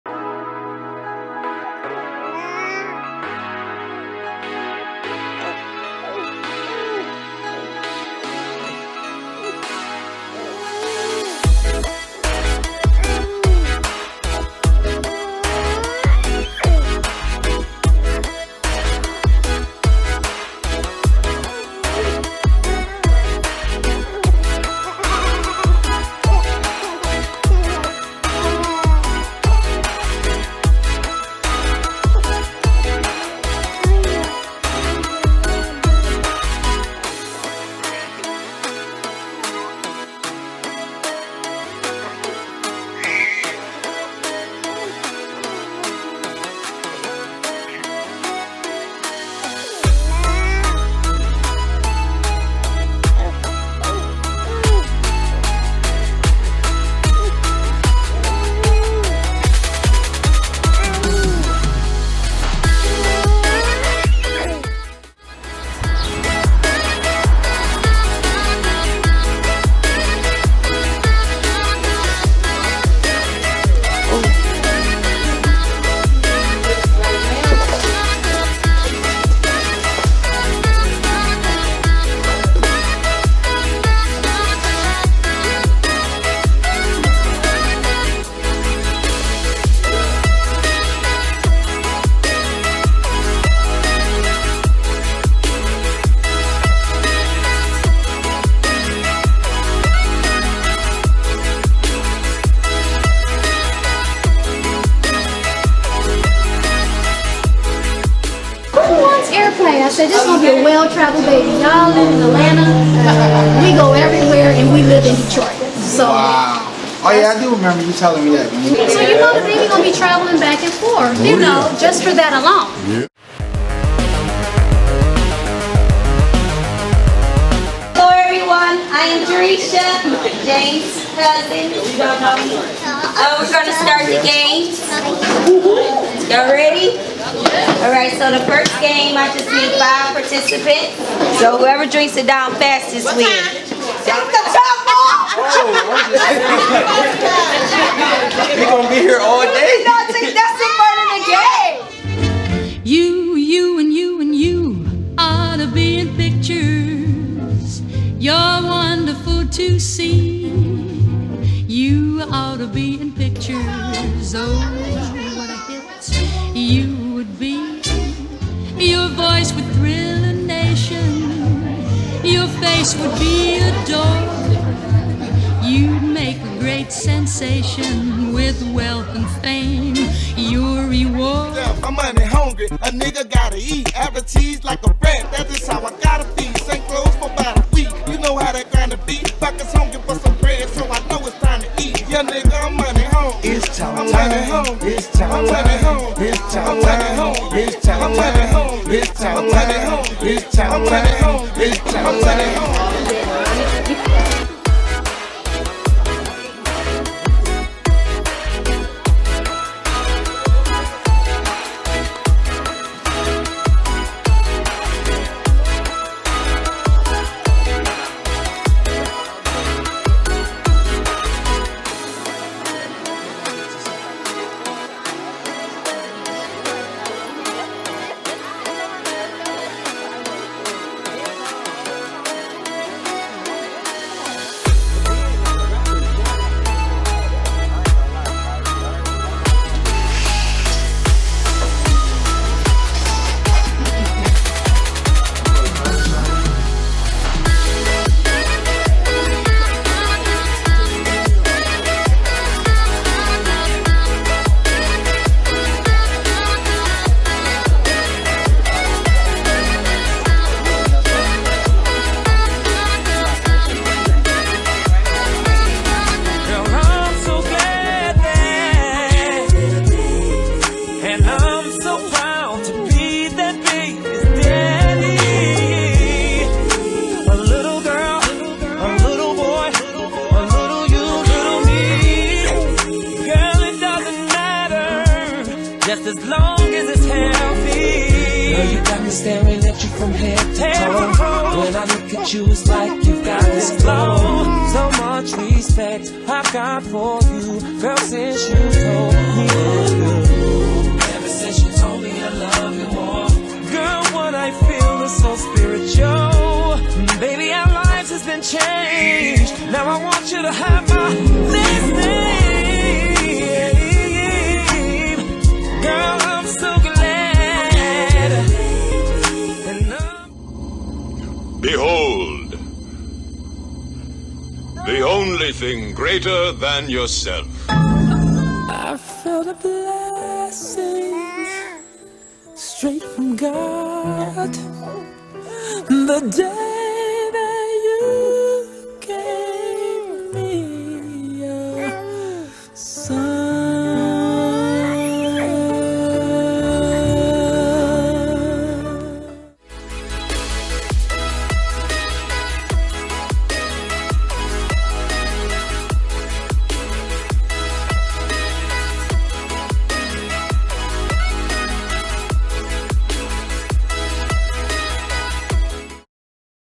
BRIDGET BABY SHOWER
Video from the June 2019 baby shower for James and Dazshanae Bridget in the Detroit suburb of Hamtramck.  The parents-to-be are native Detroiters now living in Atlanta.  The shower was hosted by James' parents, James and Tina Bridget.  The baby, a boy, is due in mid/late July.  This is a non-commercial video production done merely for the enjoyment of family and friends (the producer of this video is the ex-husband of the aunt of the father-to-be).  There are brief clips of copyrighted music used in the video.  They have been given full and proper credit at the end.